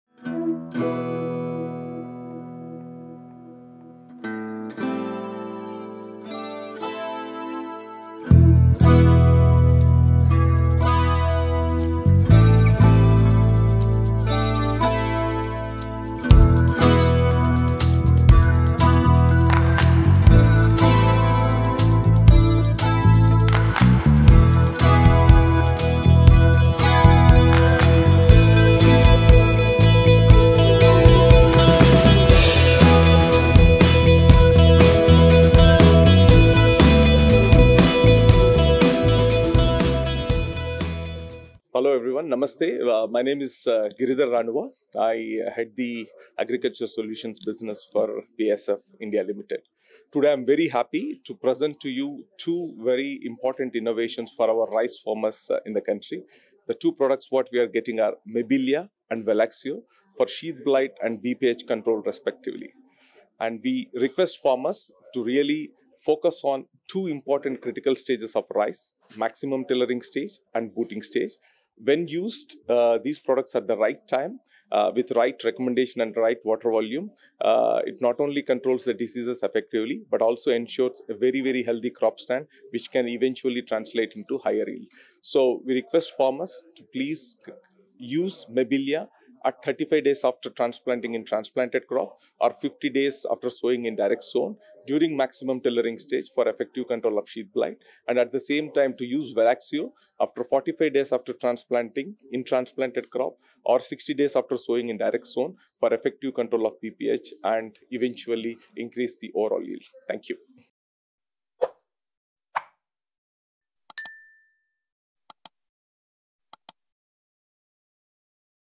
Rice Farming Solutions 2025 || No More Crop Loss? BASF’s New Launch || Valexio® & Mibelya®
BASF launches Valexio® (insecticide) and Mibelya® (fungicide) in India to help rice farmers fight pests and diseases like brown plant hopper and sheath blight.
1. Valexio®: Global-first launch in India, controls rice pests, boosts plant health & yield.
2. Mibelya®: Brings advanced Revysol® chemistry to India, offers long-lasting protection from crop diseases.

This move supports India's food security and empowers farmers with cutting-edge crop protection solutions ahead of the monsoon season.

Watch to learn how Valexio and Mibelya could change your farm’s future!